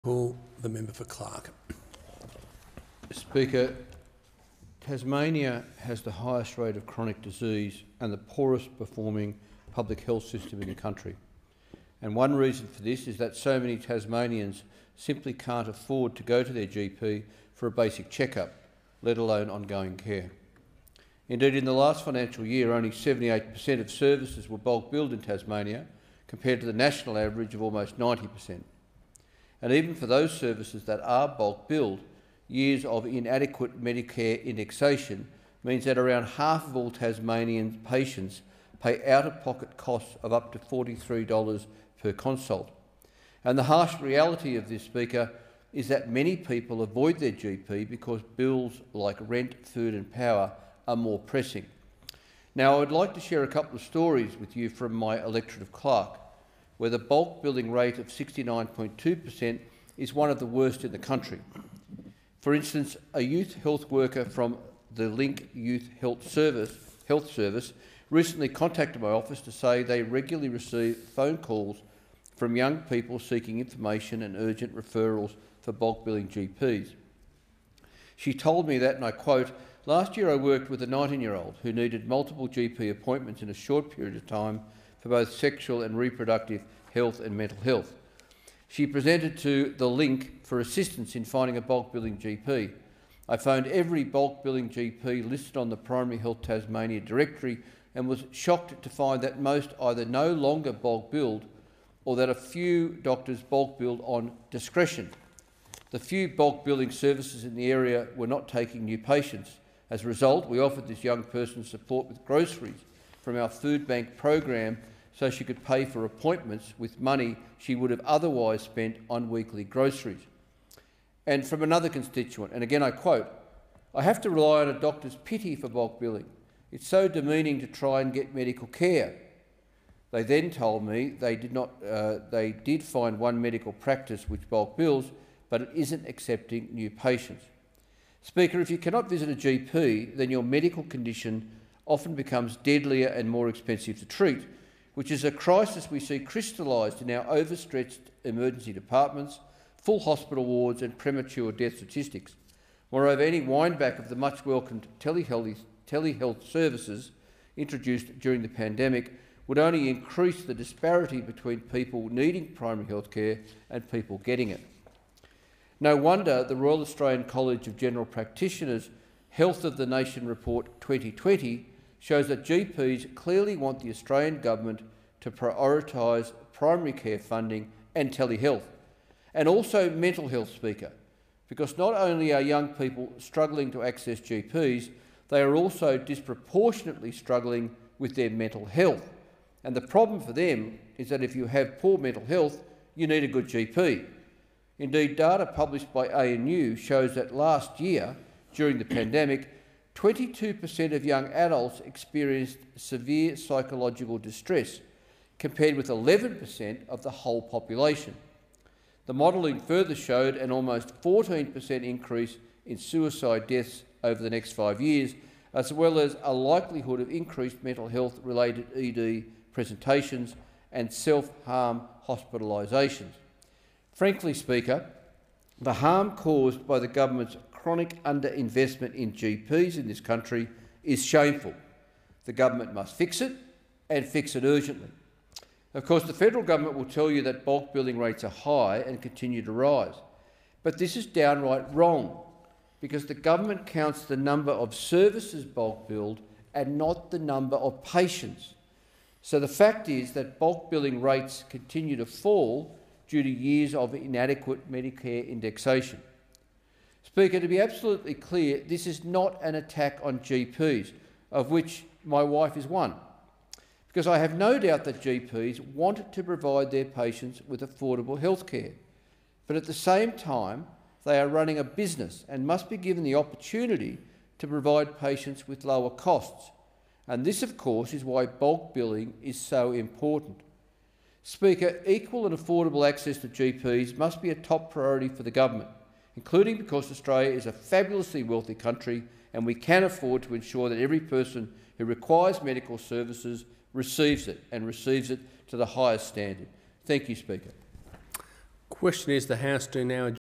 Andrew speaks in Parliament on GPs and bulk-billing
The harm caused by the Government’s chronic under-investment in primary health care, and in the GP Medicare rebate in particular, is shameful. Andrew sees this in his electorate of Clark where the GP bulk-billing rate is the 2nd worst in the country.